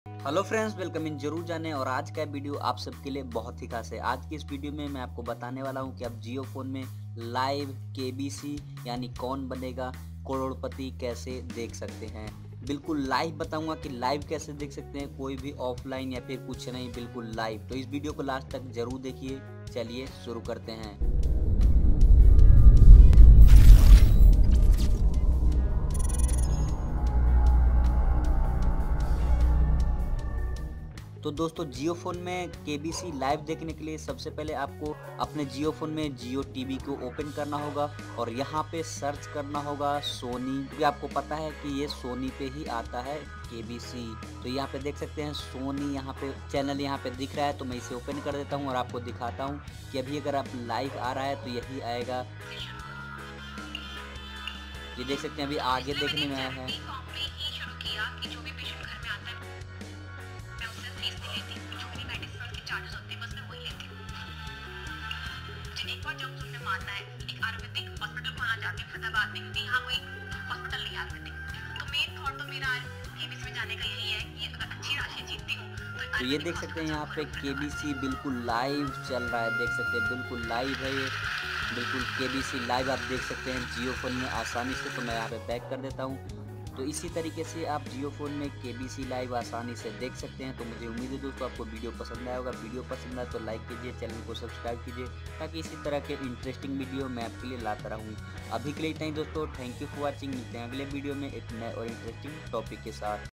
How To watch Online kbc live in jio phone !! How to Play kbc play alone in jio phone !!
hello frienda welcome in jrur jane

aaj ke is video me mai aapko ye batane wala hu ki aap apne jio phone me kbc live kaise dekh sakte hai aur kbc play alone khel
sakte hai  (How To  watch Online  kbc live in jio phone !! How to Play kbc play  alone in jio phone !!)

video ko pura jrur dekhie video pasand aaye to video ko